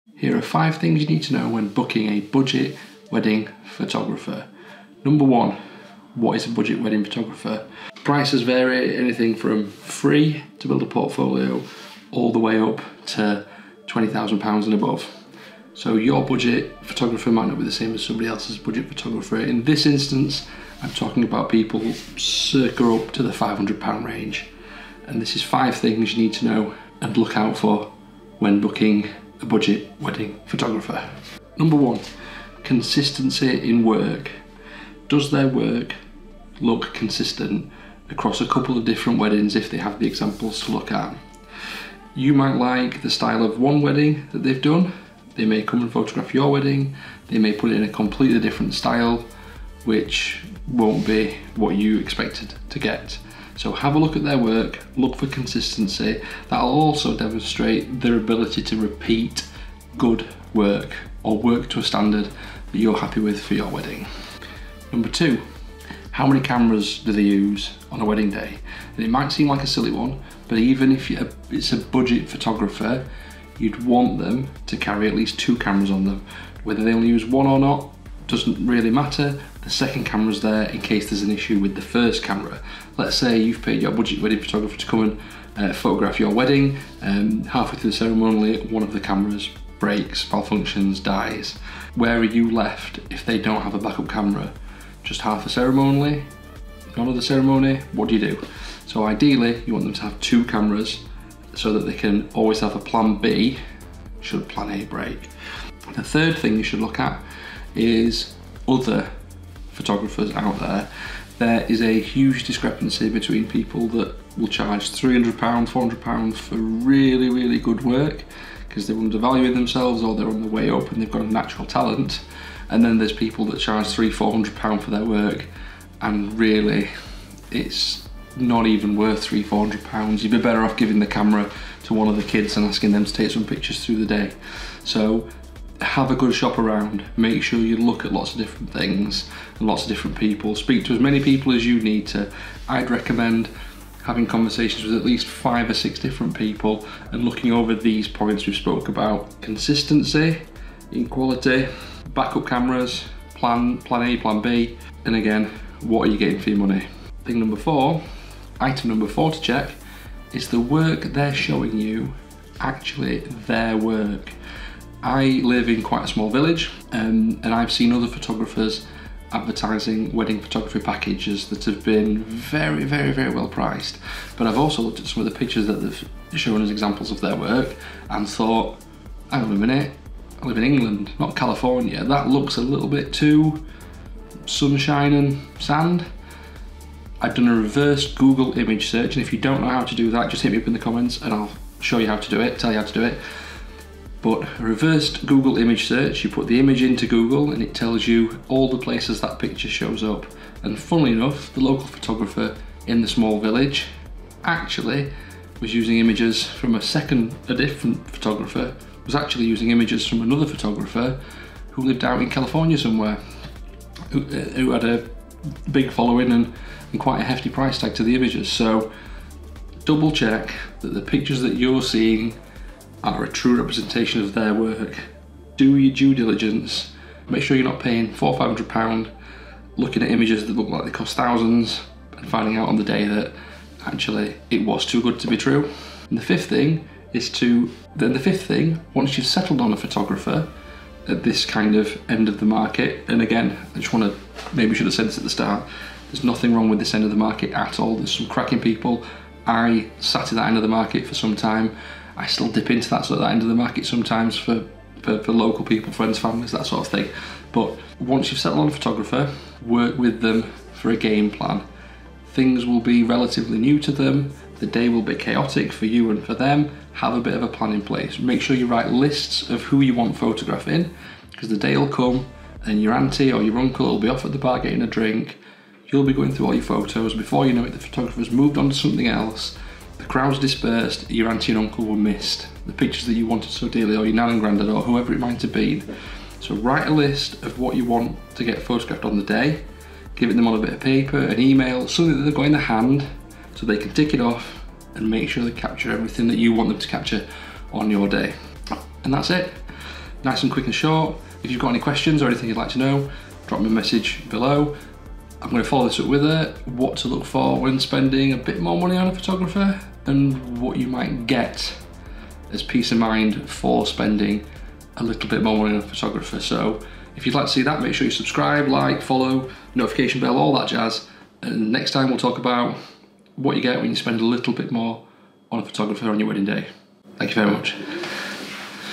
Finding your Budget Wedding Photographer
In this video, I'll speak about what to look for when booking a budget wedding photographer (Under £500) for your wedding, so you get the best bang for your buck.

If you like this video, make sure you follow to catch the next one. what spending a little more money buys you in wedding photographers and the peace of mind